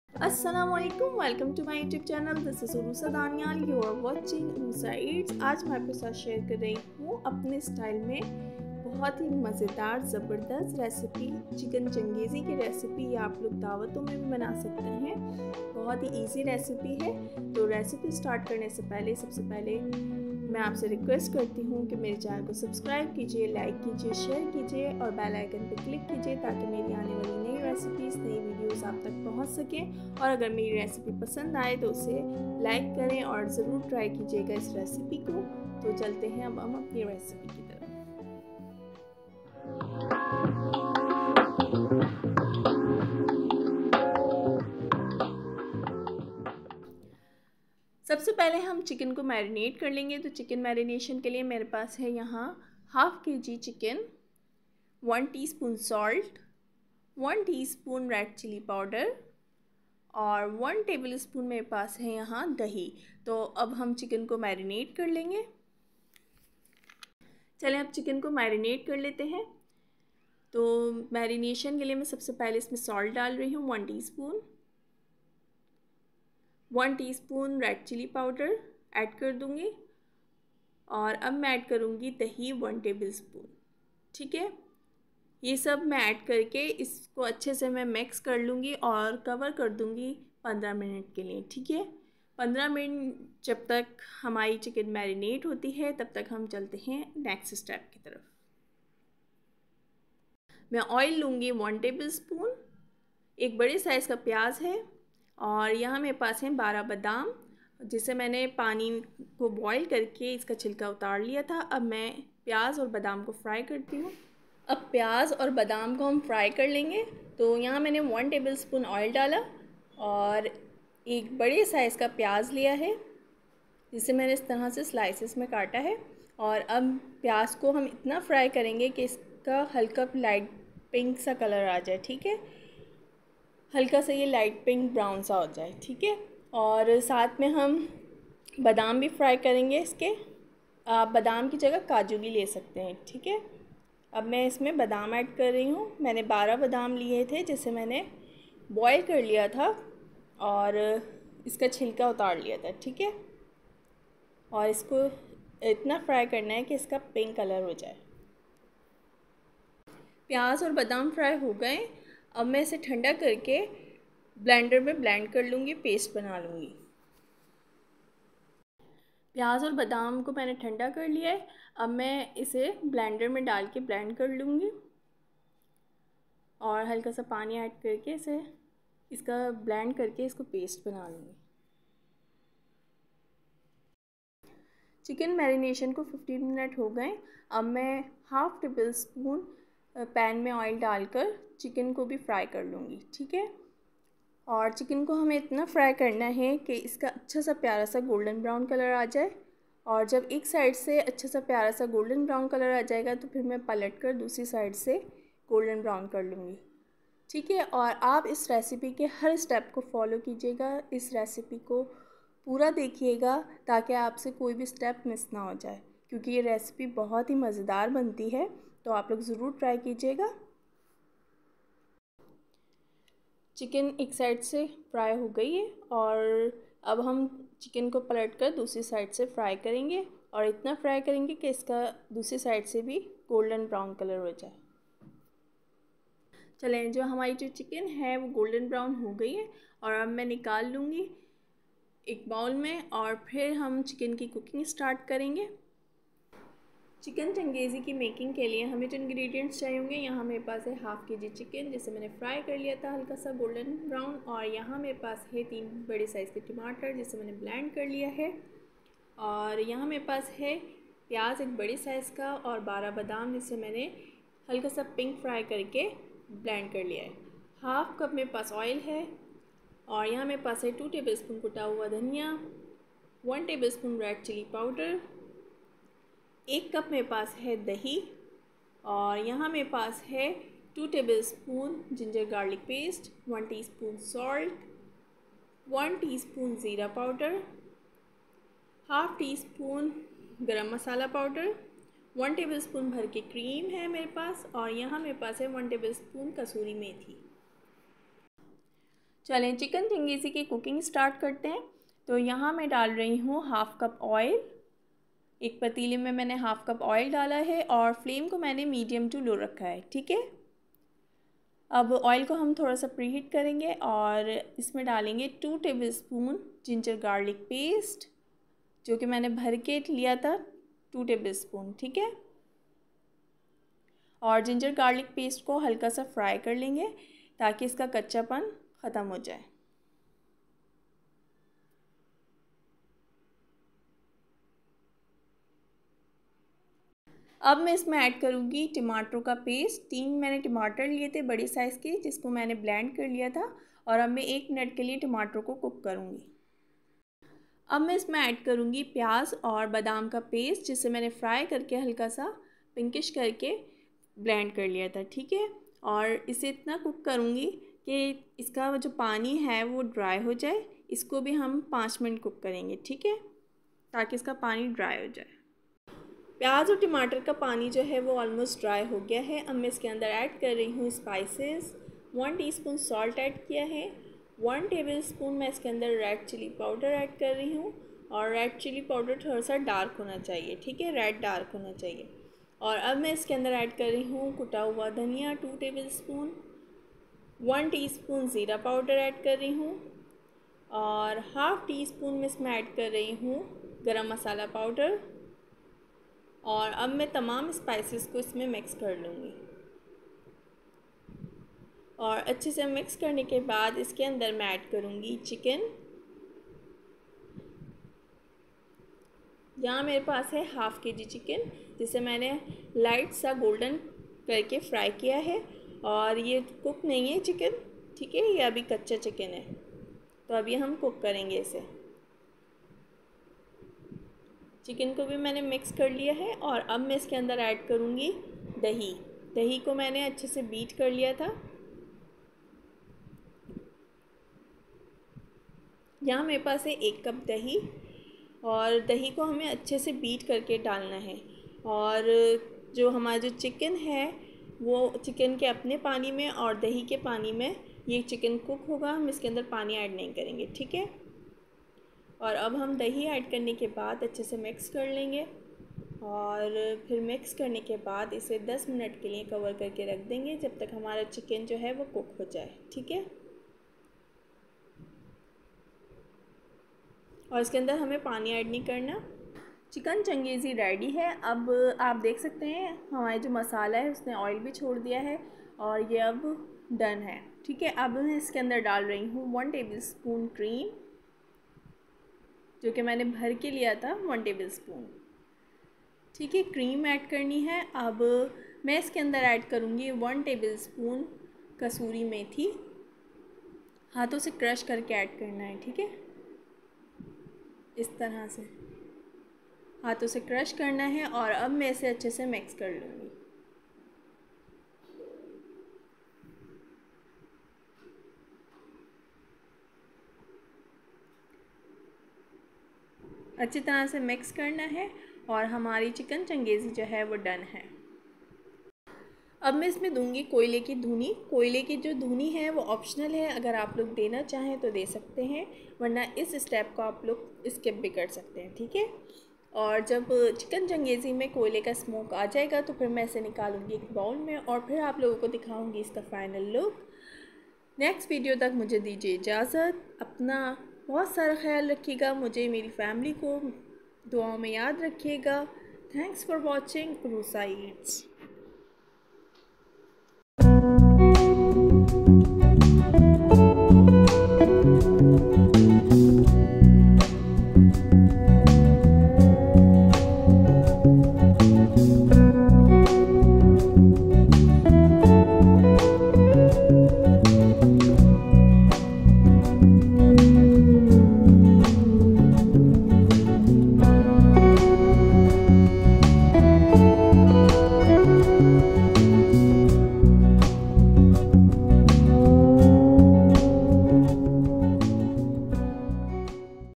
Chicken Changezi Recipe By Uroosa Daniyal | Uroosa Eats | Recipe # 8
Chicken Changezi, a staple of the Mughlai cuisine, is a favorite among foodies. Chicken Changezi is very well-liked in the northern regions of India. The dish's distinctive taste comes from the recipe's hidden components, which have garnered it broad praise among connoisseurs.

Chicken Changezi can be made in under 30 minutes and is delicious with hot pita or rice. Try your hand at cooking this non-vegetarian stew and you'll be the talk of the dinner table.

It's going to be so good that you and your loved ones will be licking your fingers the whole time. Whether you're having a birthday party, anniversary party, or a party for your family, this meal is guaranteed to be a hit.